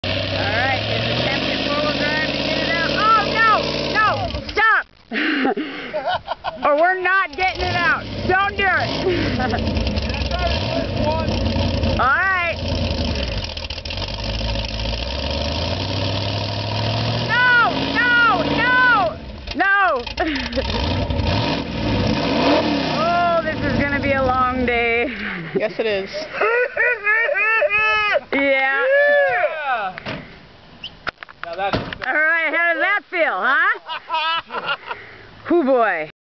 79 Bronco hittin a badass mud hole - "This is gonna be a long night!"
Moses Lake Mudflats. This is part 2... The front end went down like the Titanic! This is what we call "Hank Stuck"!! It took 11 hours of diggin, pullin, diggin, and yes, more pullin. The Bronco 4wd let go during one pull. 3 other trucks were damaged in this pull (sorry peoples...) and finally was winched out late in to monday night. As was said in part one, again, thanks to everyone who busted ass to help get this truck out. Again, thanks Baby Brother!